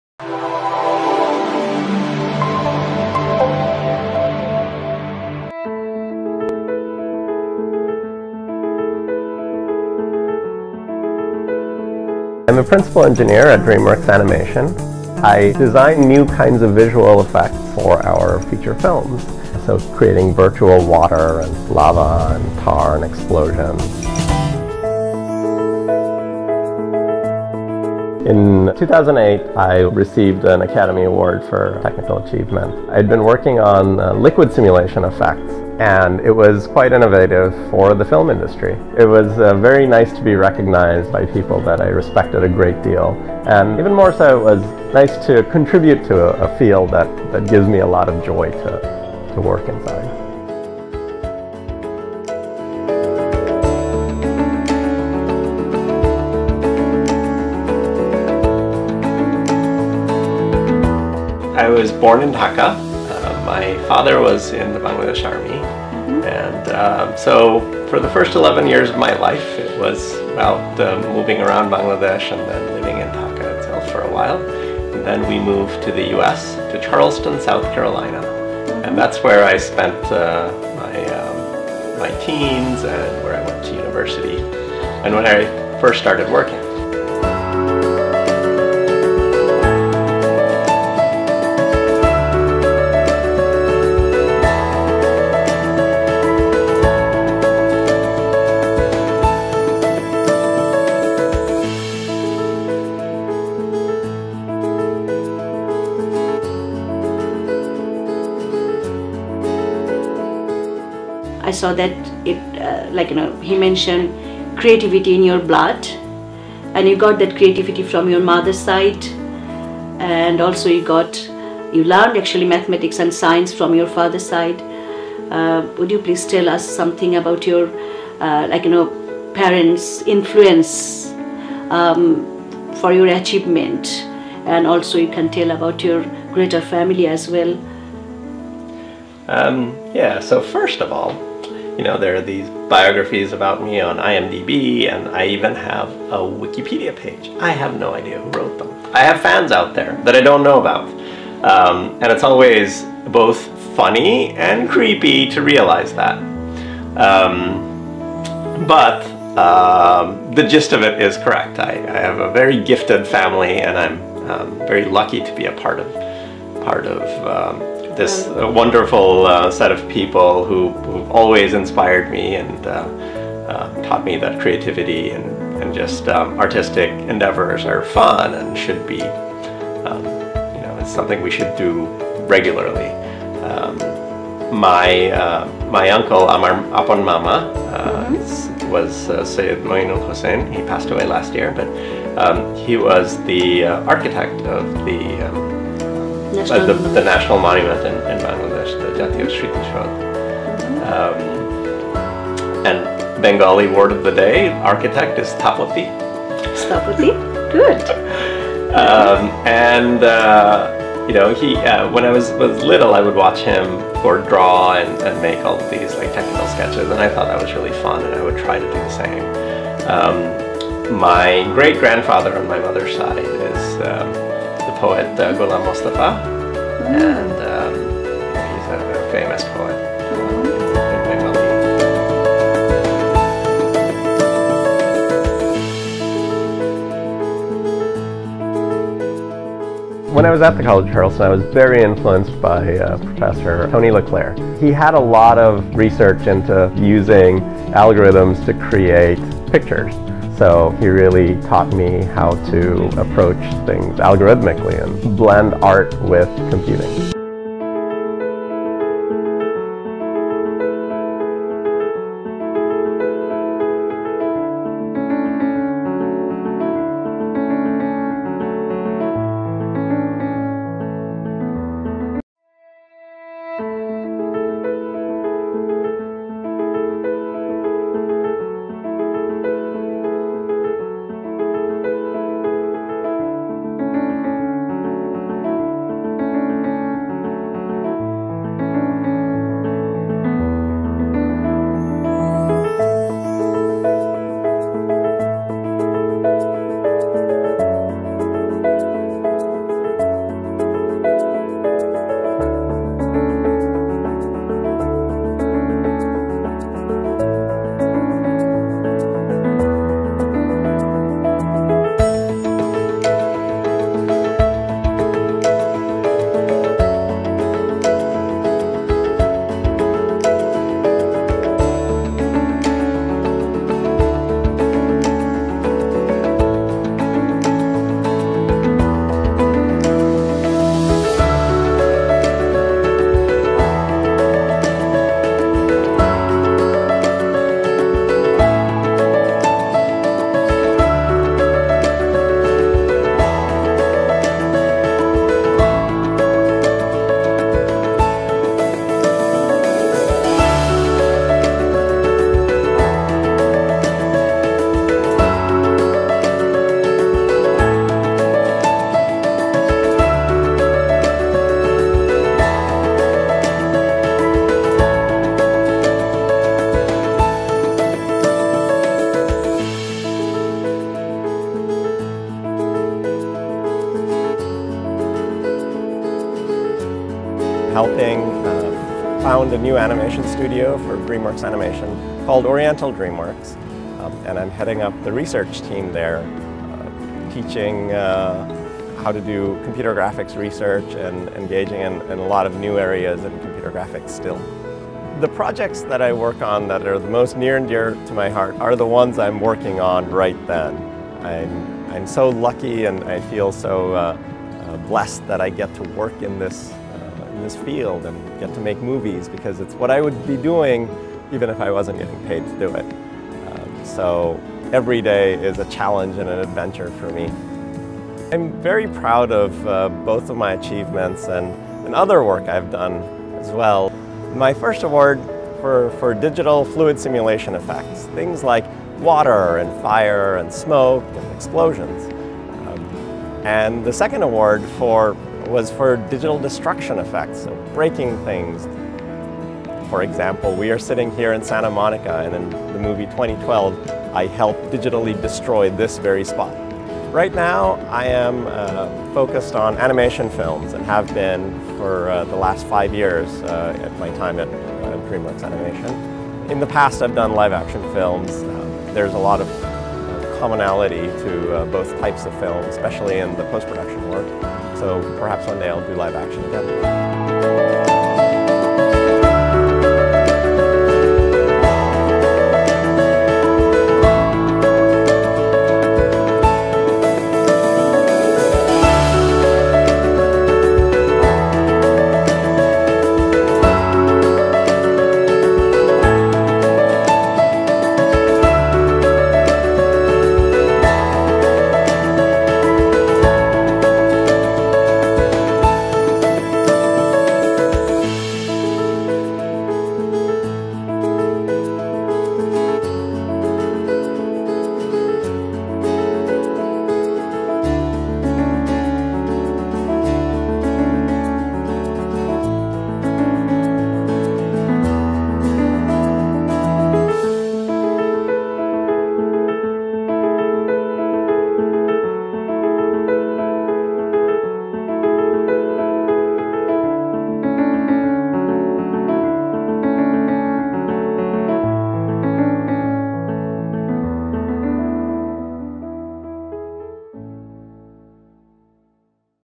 The Enlighten One - Nafees Bin Zafar
Nafees Bin Zafar is a Bangladeshi software engineer who received a Scientific and Technical Academy Award (OSCAR) in 2007 for his contribution to the development of the fluid simulation system for the movie: Pirates of the Caribbean: At World's End, thus becoming the first Bangladeshi to win an Oscar.

Now he is working on a new destruction technique. At the end of the day animation is Computer Programming. But this requires a lot of creativity. Creativity is in Nafees's blood.